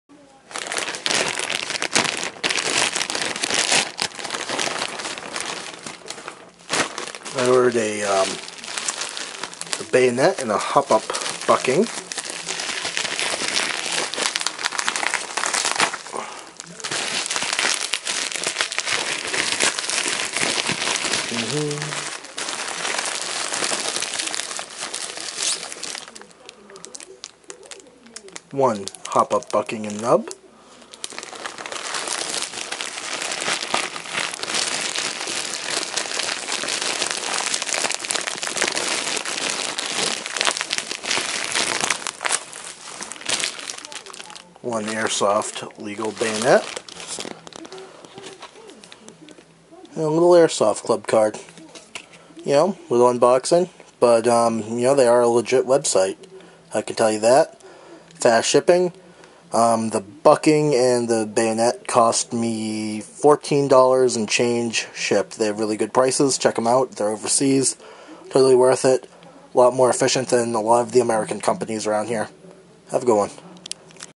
Airsoft-Club Unboxing.....well unbagging
I ordered an Airsoft Legal Bayonet for my many M4/16 variants and a Gaurder Hop up bucking.
I made the order 11 Days ago and got it now.  Airsoft-Club is located in hong kong, they have great prices and fast shipping.

Total was about 14.63 for both items and shipping